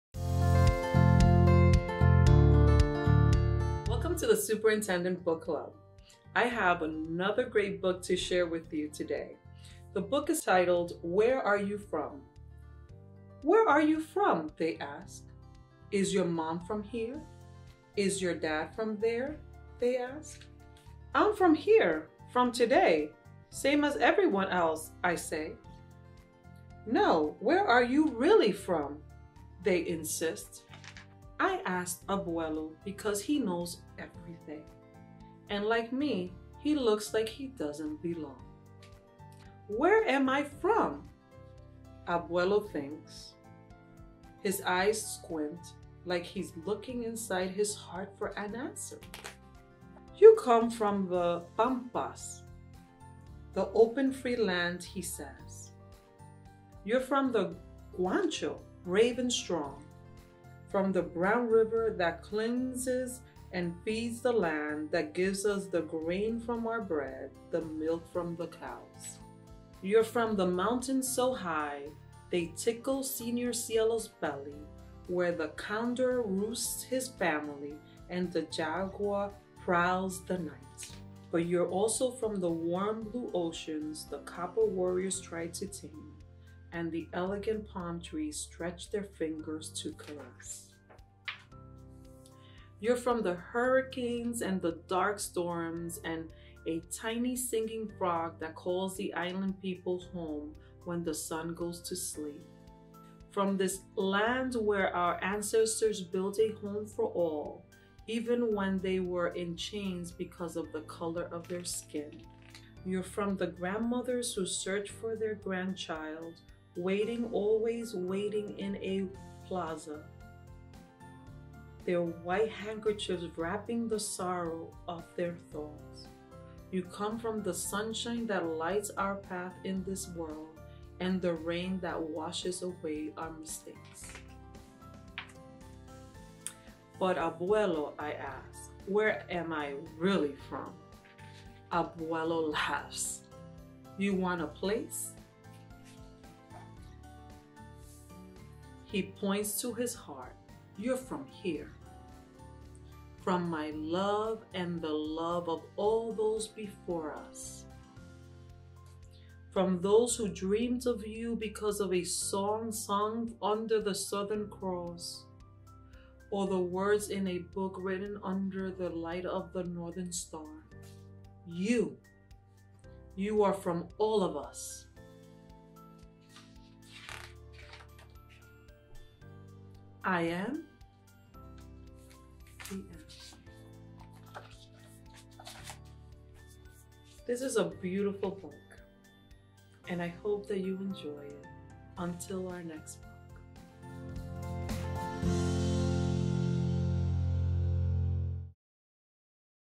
Dr. Estrella's Book Club: Where Are You From?
Celebrate ReadAcrossAmerica Day, and the start of WomensHistoryMonth with the Superintendent’s Book Club pick, “Where Are You From?” by Yamile Saied Méndez, illustrated by

Until our next book, please enjoy!